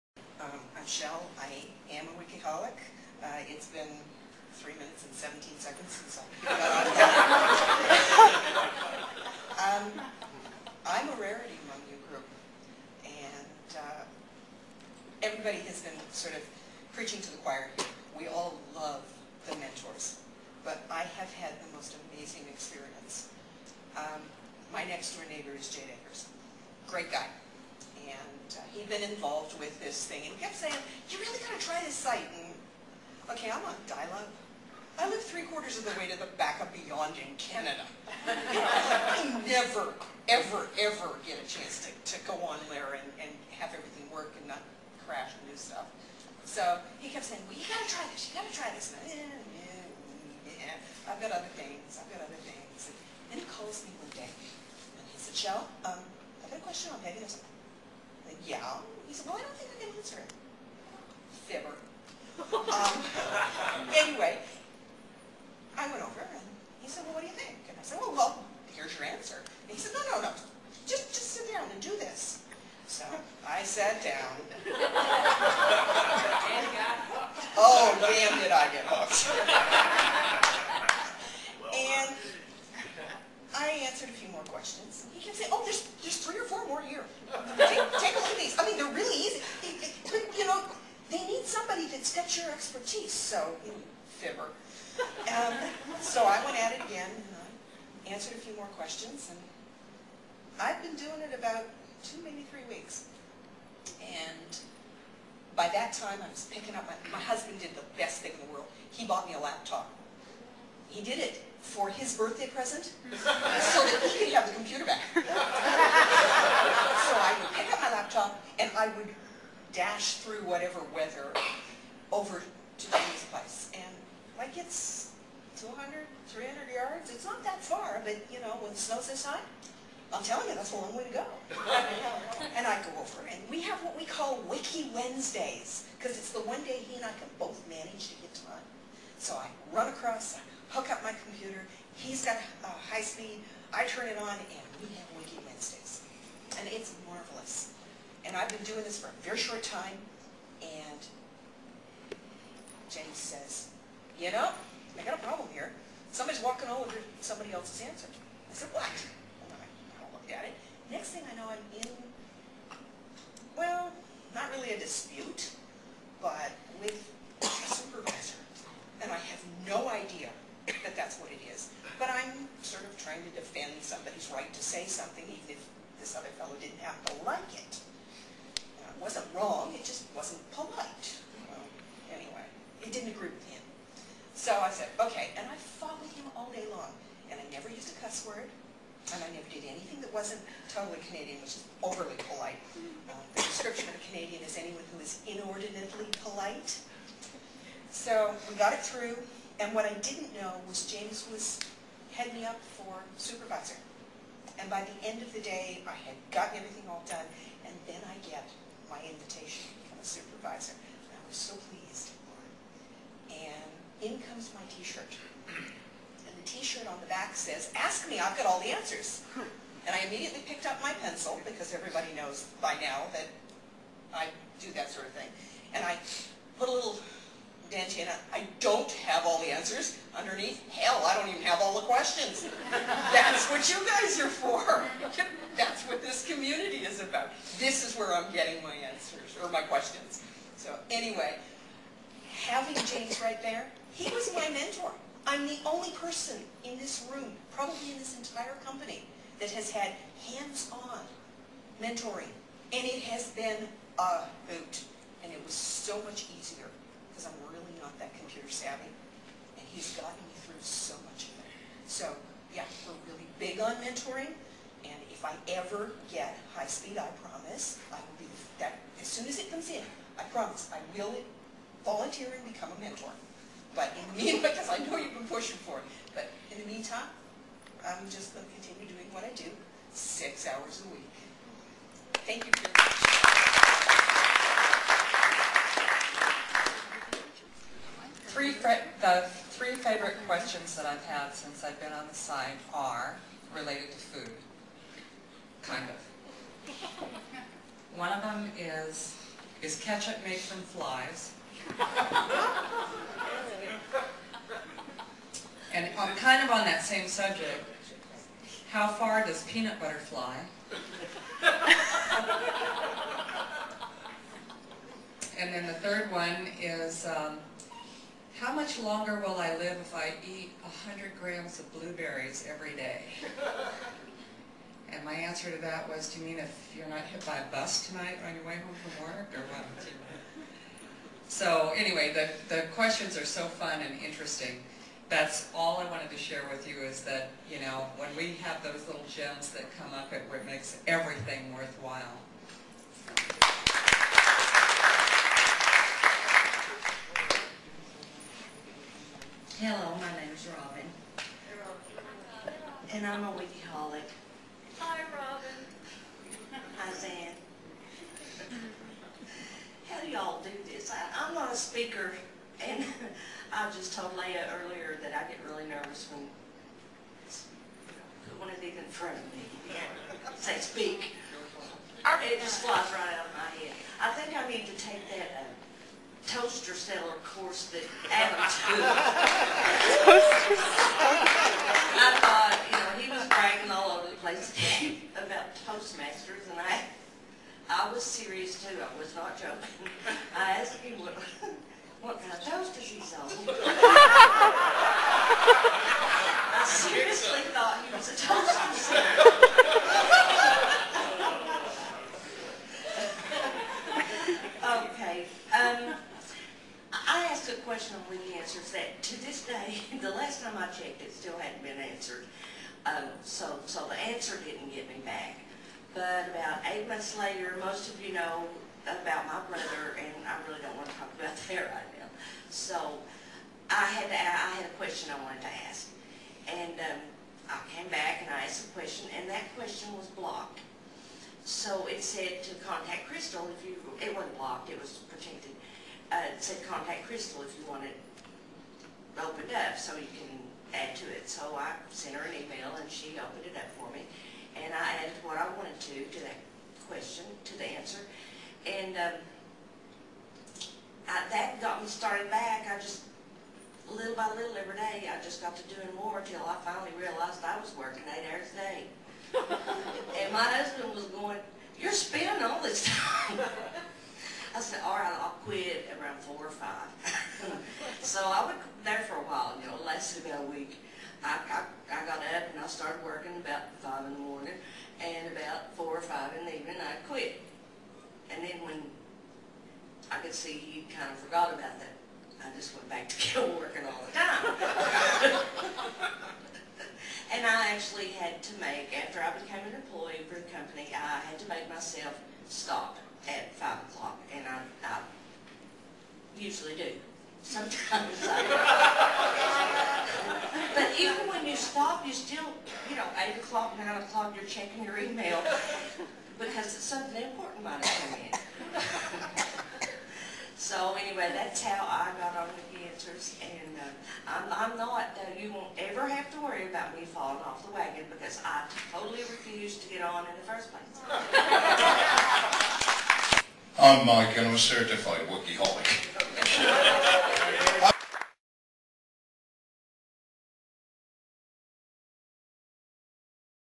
Answers.com Summit: Confessions of a Wikiholic (4/4)
Wikiholics: They are all around us. They ARE us. And you know what? We're ok with that! Hear the true stories and confessions of Wikiholics at the Answers.com Summit 2009. Part 4 of 4.